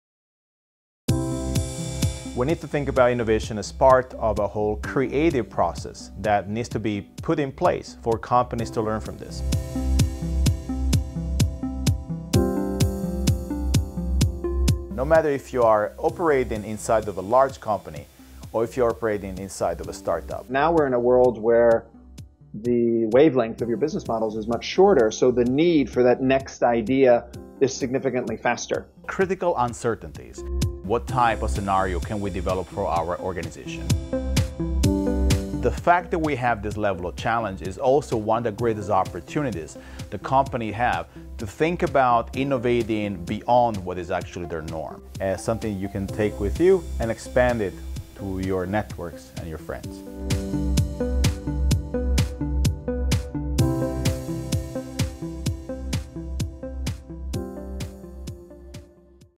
Innovation for Growth HiOP - Trailer
You will gain insight to the various degrees of leadership that allows innovation to be harnessed and nurtured in organizations. This course will push your understanding of what defines leadership in today’s world. By joining this course, you will hone your understanding of innovative leadership